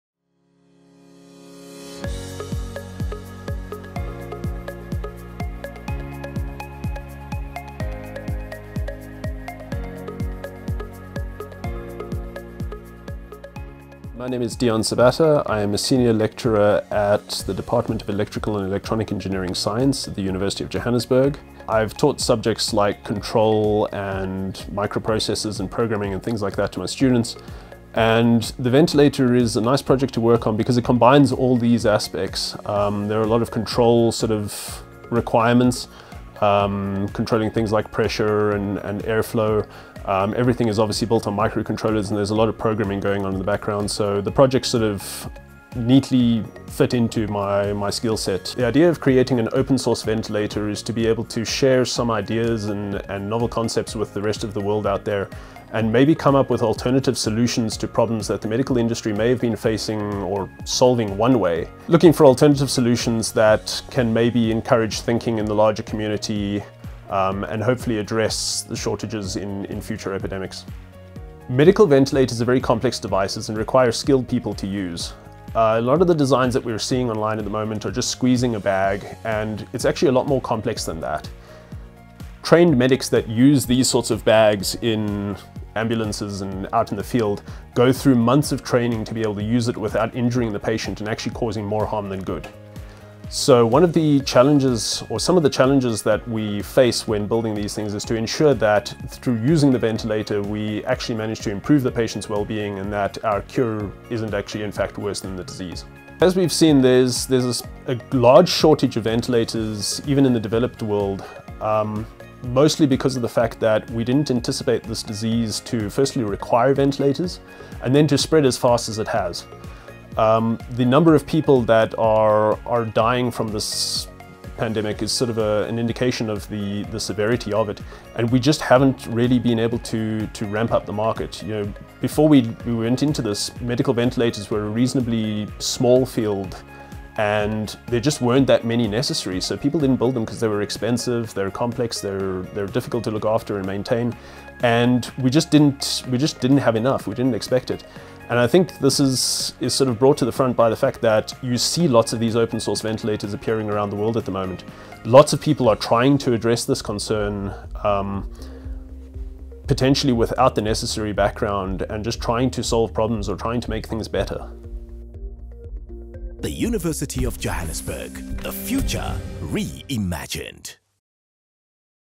University of Johannesburg prototypes cost-effective ventilators
A multidisciplinary team of engineers and healthcare practitioners at the University of Johannesburg (UJ), identified several simple, safe and scalable open-source ventilator designs that could meet the strict specifications for use with patients if futher developed and tested. These designs will support the development of the critical control systems that protect a patient supported by a ventilator.